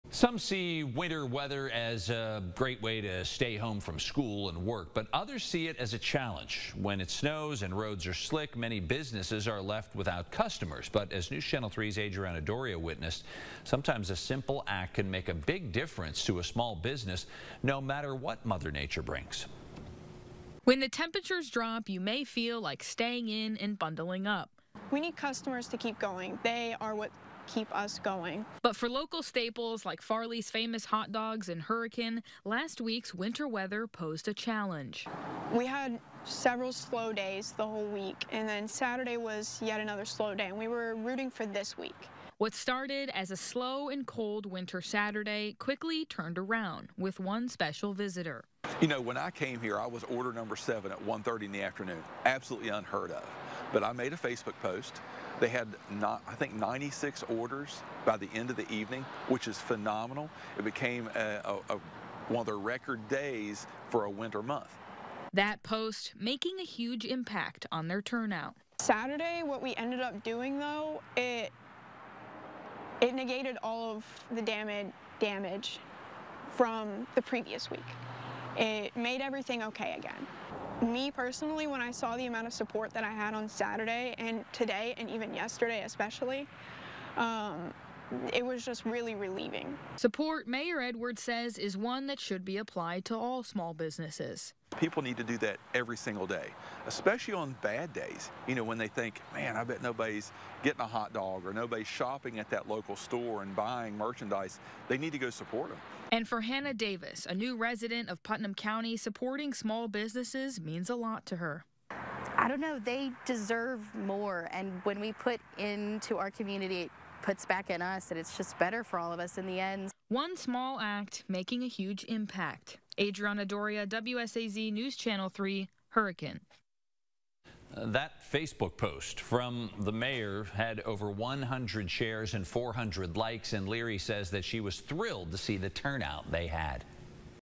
Supporting local businesses in winter weather: ‘We need customers to keep going, they are what ke...
Supporting local businesses in winter weather: ‘We need customers to keep going, they are what keep us going’